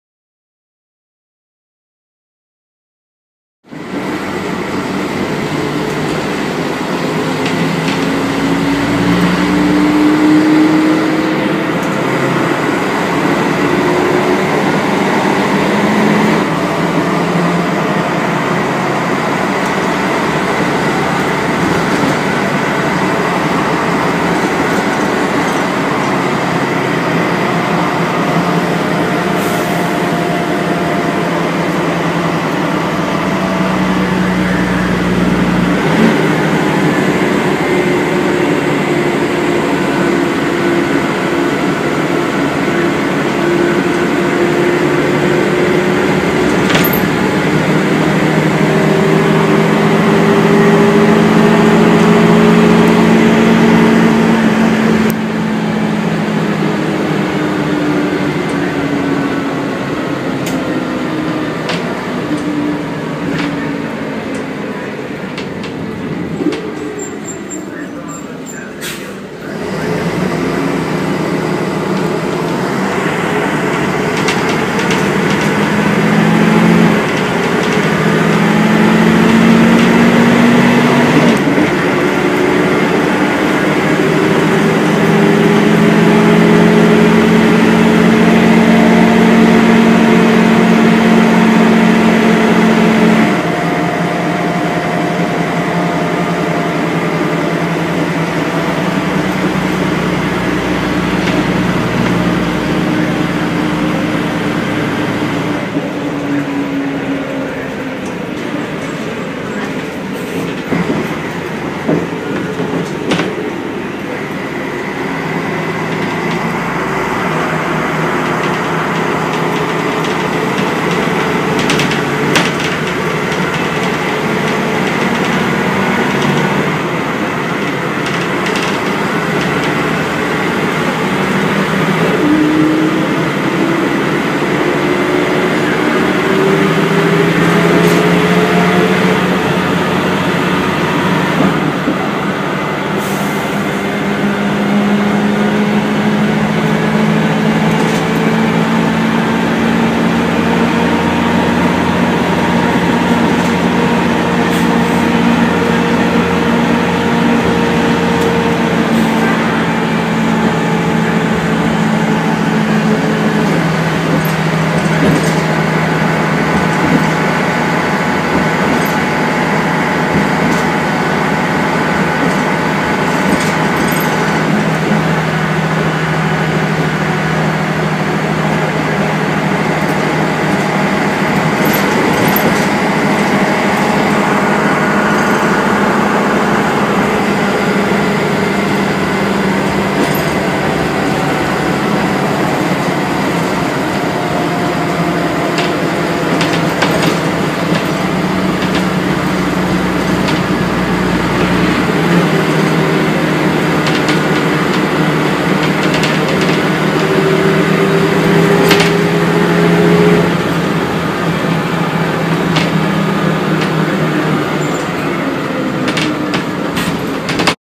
Veolia Transport NSW Bus 94 (Volvo B10M - PMC)
Taken 27/4/13 on East Hills line rail bus duties between Padstow and Kingsgrove. This bus was built in 1989 and has recently been fitted with a new front, which looks weird to say the least. This one is ex Southtrans.